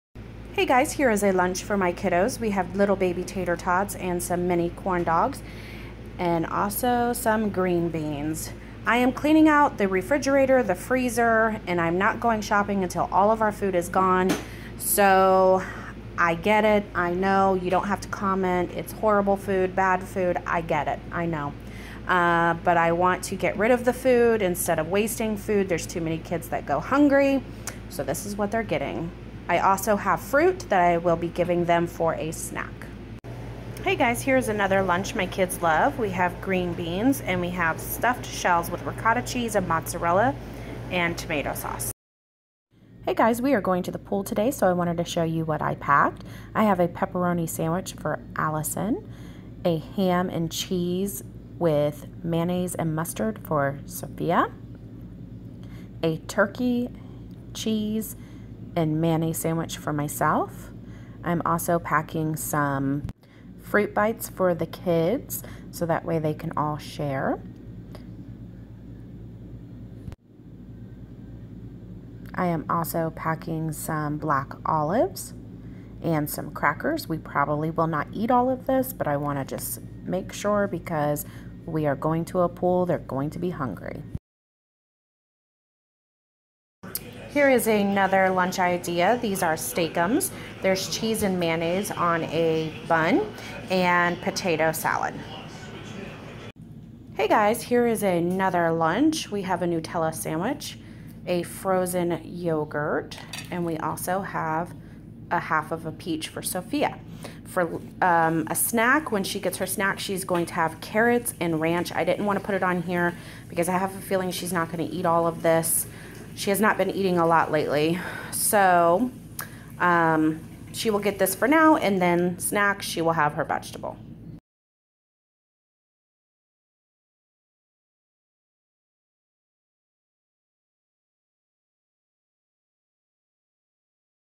Kids summer lunch ideas for the week
Here are 5 summer lunches for kids or adults!!